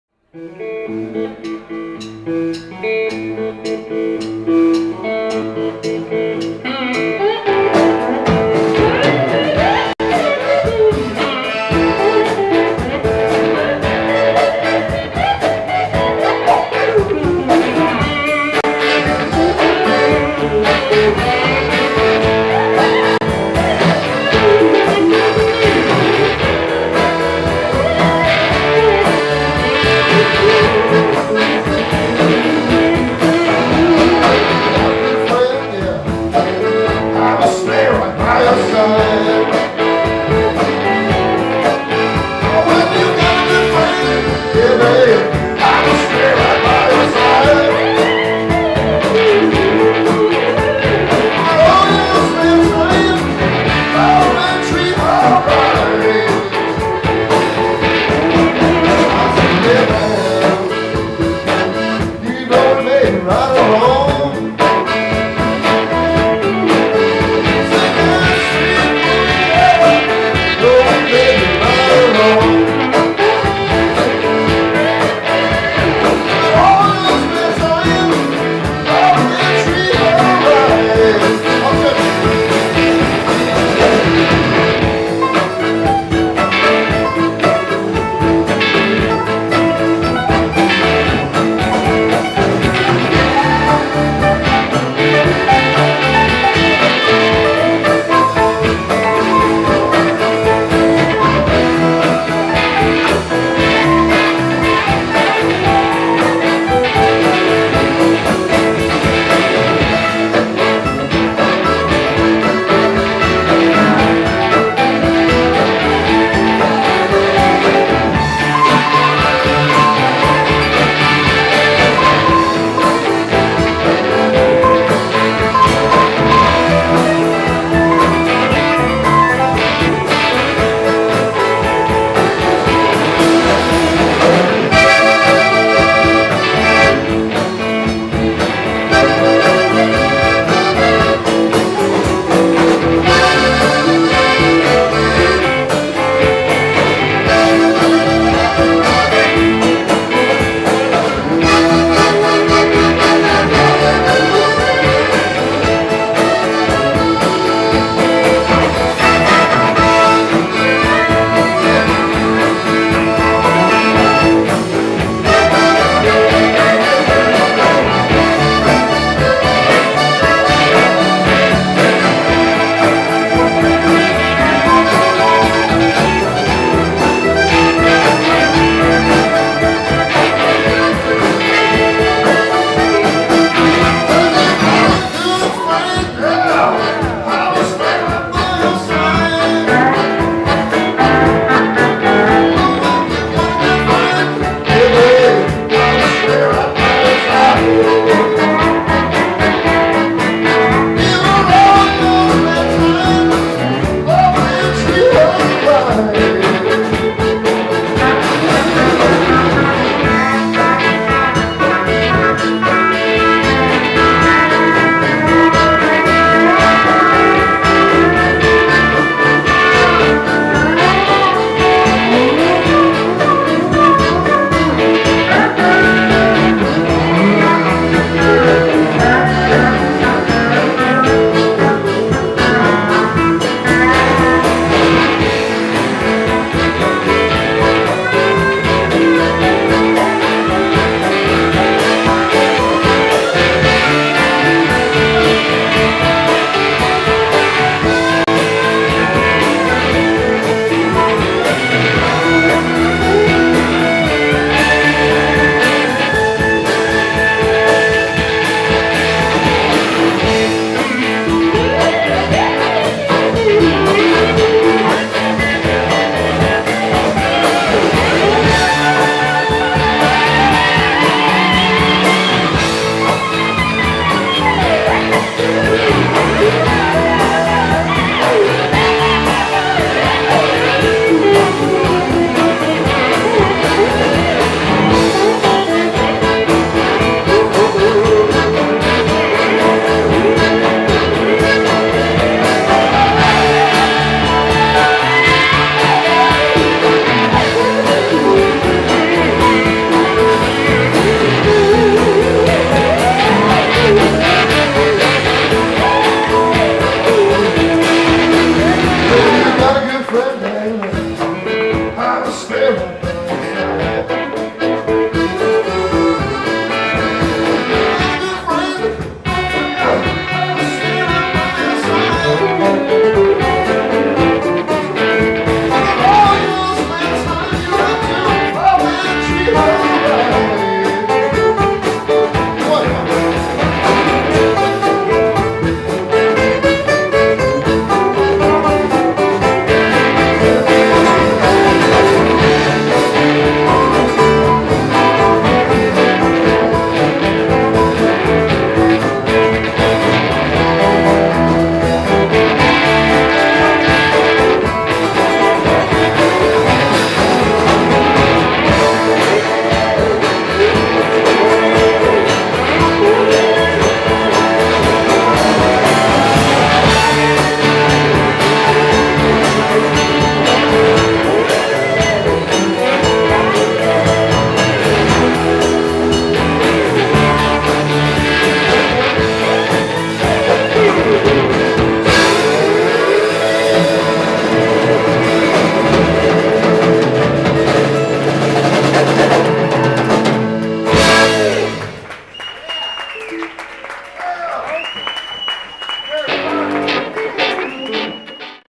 Buddy Cage, Kerry Kearney, Michael Falzarano, Professor Louie at Tribeca Blues, N.Y. 2001 Part 4
Buddy Cage, Kerry Kearney, Michael Falzarano, Professor Louie at Tribeca Blues, N.Y. 2001 Part 4 Video produced by Larry Blumenstein Video Productions.